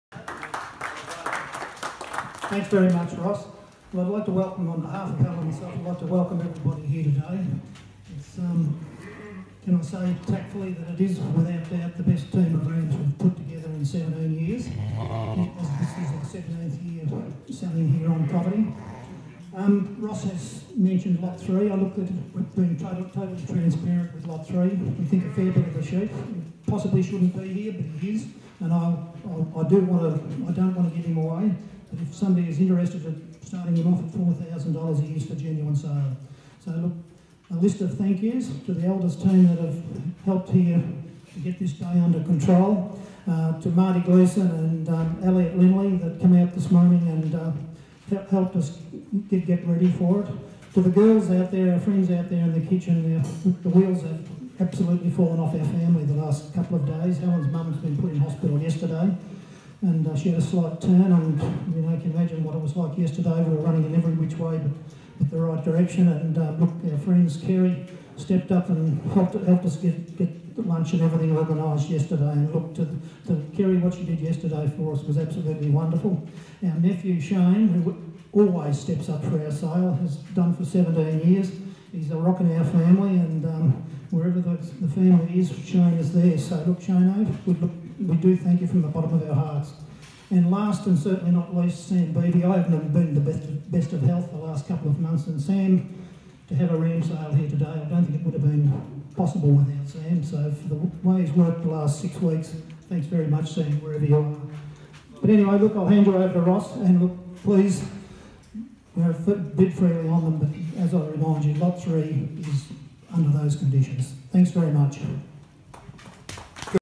Best ever lineup at Montrose Hill
Montrose Hill stud co-principal George McKenzie welcomes the crowd to the stud's annual on-property ram sale, of which he said offers the best ever lineup.